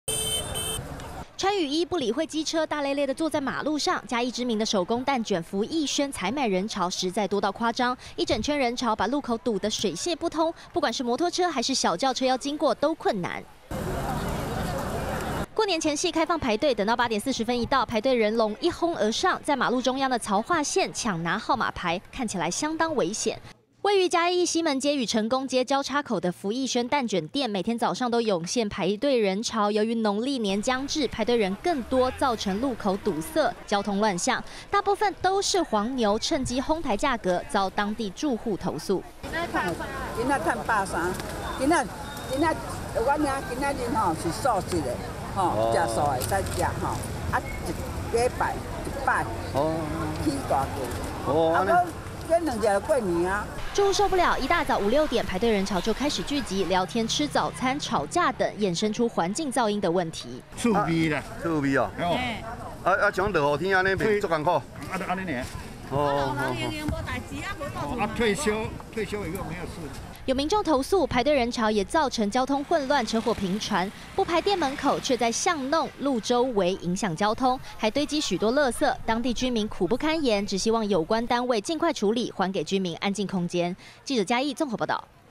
20190121中天新聞 排隊人龍衝車流搶號碼牌 蛋捲黃牛引民怨
年節將至，伴手禮店家卻惹民怨，這嘉是為在嘉義的福義軒蛋捲店，遭當地住戶投訴，每天一早六點就開始湧現排隊人潮，將四周的巷子及路口擠得水洩不通，裡頭不少都是要把蛋捲轉手提高價格出售的黃牛，甚至八點多開始發號碼牌時，民眾一擁而上，在馬路中央的槽化線搶拿號碼牌，畫面相當驚險，加上吵雜的喧鬧聲，讓附居民苦不堪言。